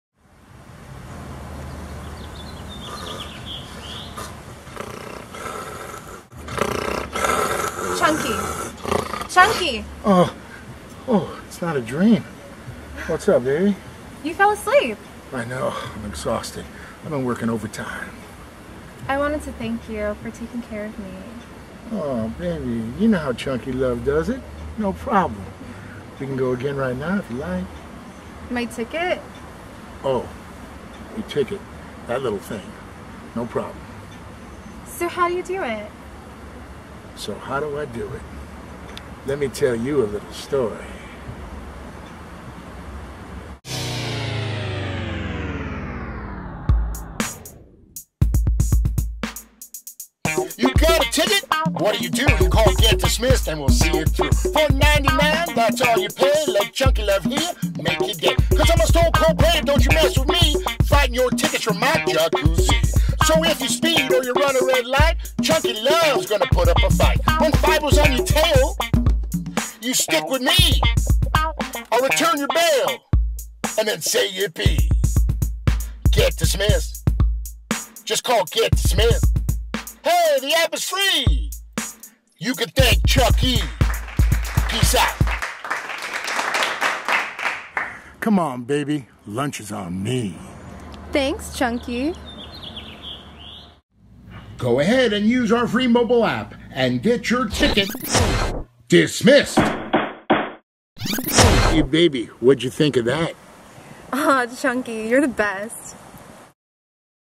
Chunky Love Raps GetDismised
Brad Grunberg, the GetDismissed Man, aka Chunky Love is the man.

Just chillin at the pool sleeping, gets woken up by his hot bikini baby.  All she want to do is thank Chunky for getting her out of a red light camera ticket.

Chunky gonna tell her how it’s done, cuz you know how Chunky Love does it, no problem.  Chunky Love raps GetDismissed to explain.

Go ahead and listen to Brad Grunberg, the GetDismissed Man, aka Chunky Love rap.

Join us every week to see what Brad Grunberg, the GetDismissed Man is up to today.